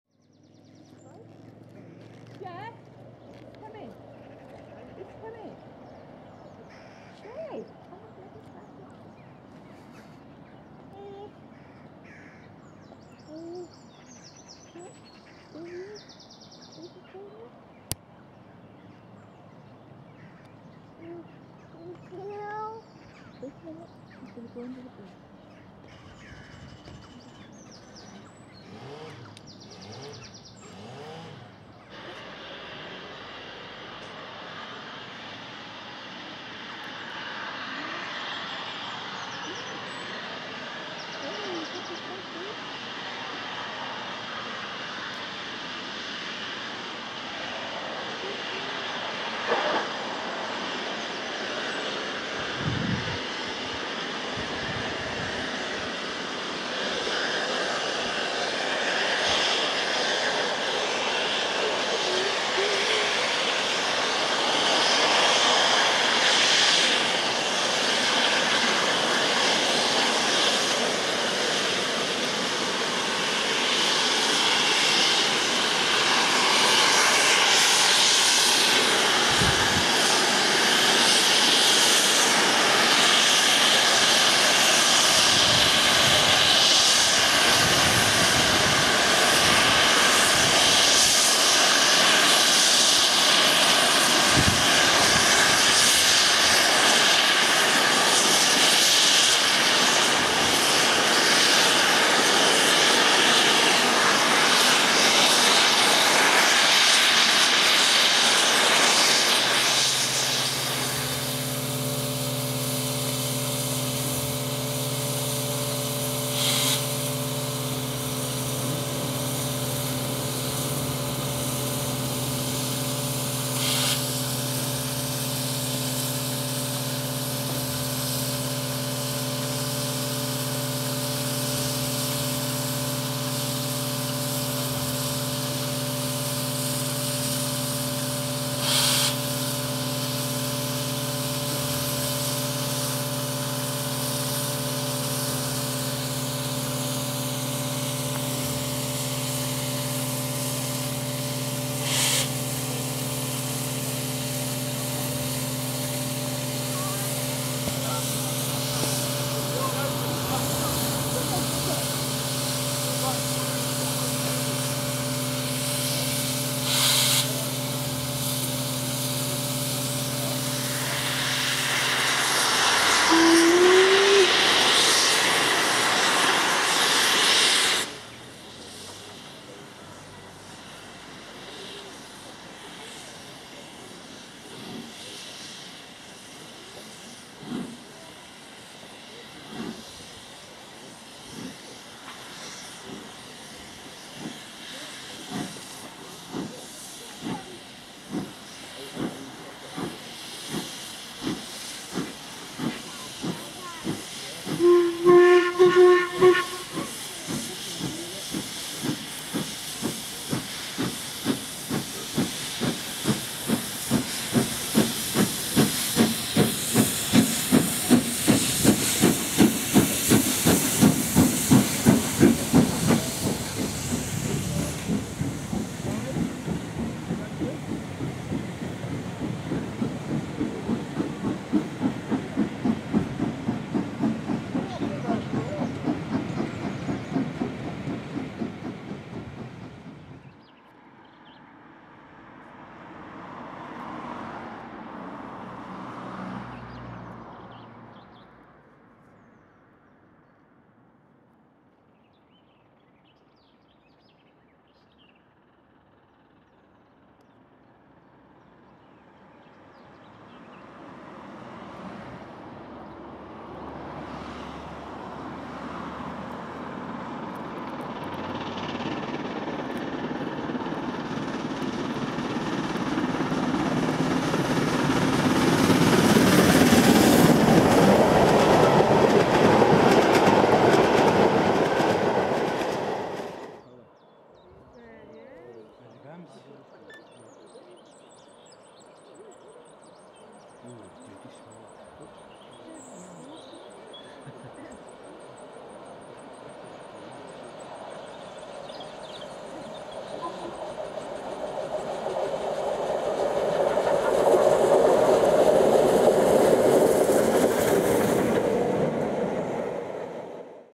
The Red Dragon - 6201 Princess Elizabeth (19th June 2010)
The Red Dragon tour from Solihull to Swansea on the 19th June 2010 run by Vintage Trains Limited.
A lot of the film is quite shaky in places as it was all done handheld with some support from the bridges.
This was my first time using the camcorder (Panasonic SD60) as well as filming trains so it was a voyage into the unknown for me.
Not a great deal of footage taken on the day but I've put in most of what I did get.